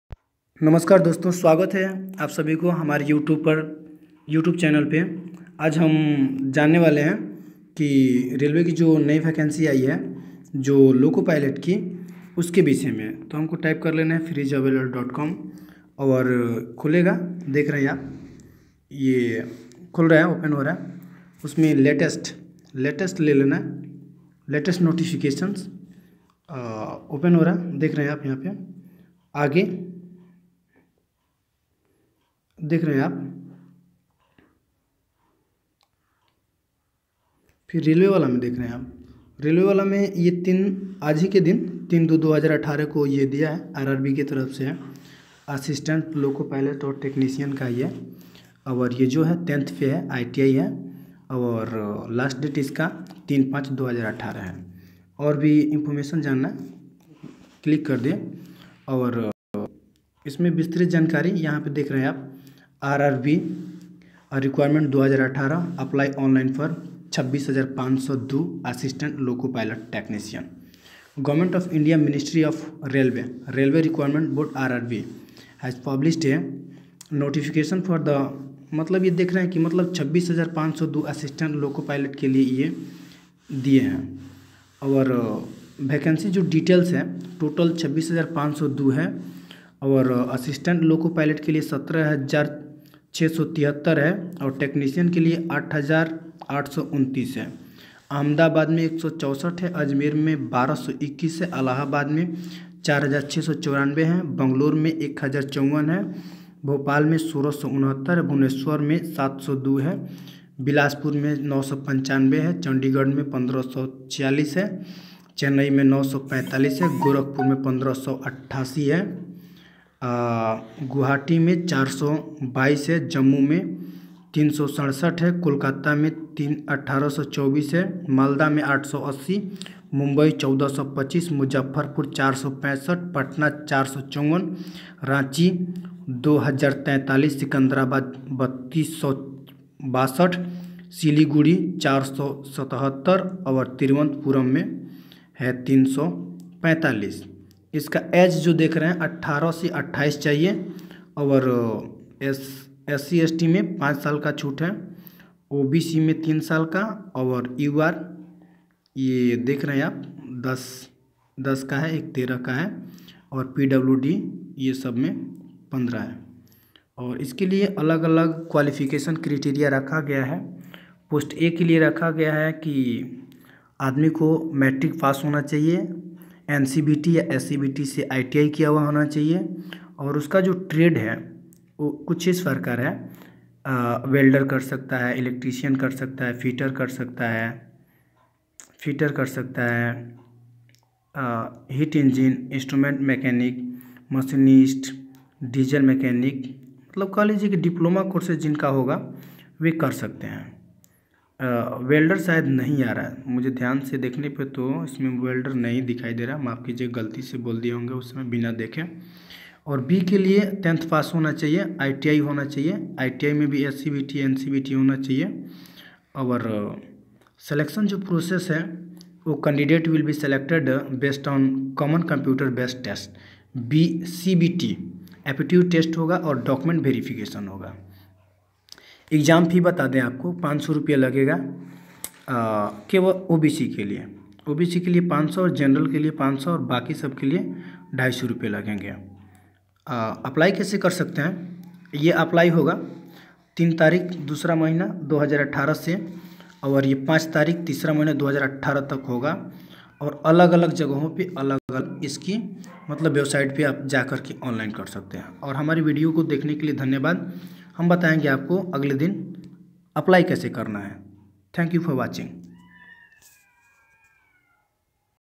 How to get railway recruitment notification 2018
Namskar aap railway ke notification ko Dekh w download mar payege